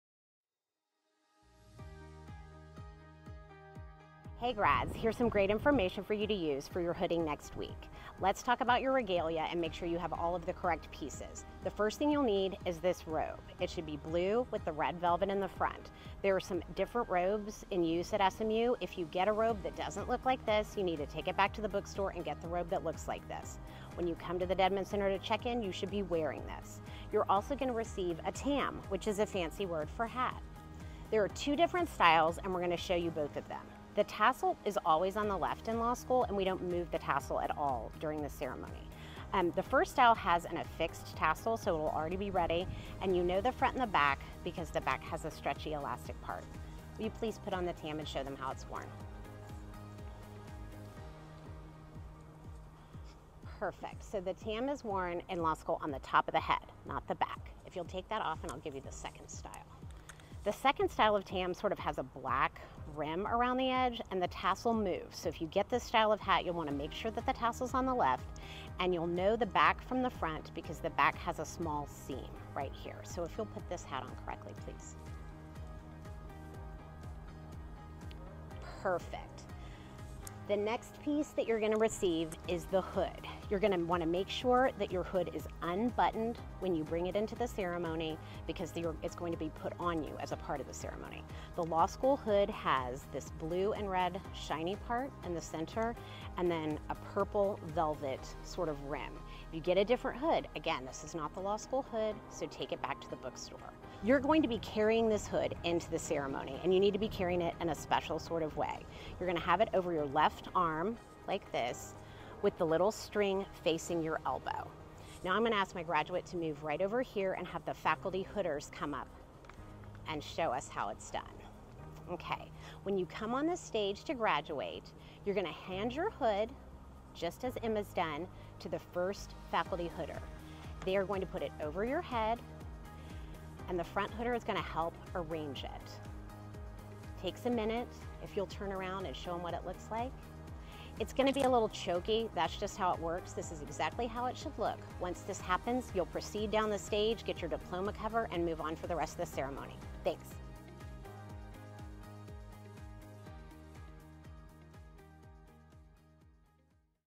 How to wear your SMU Law graduation regalia
Learn more about how to wear your robe, tam, tassel, and hood for graduation.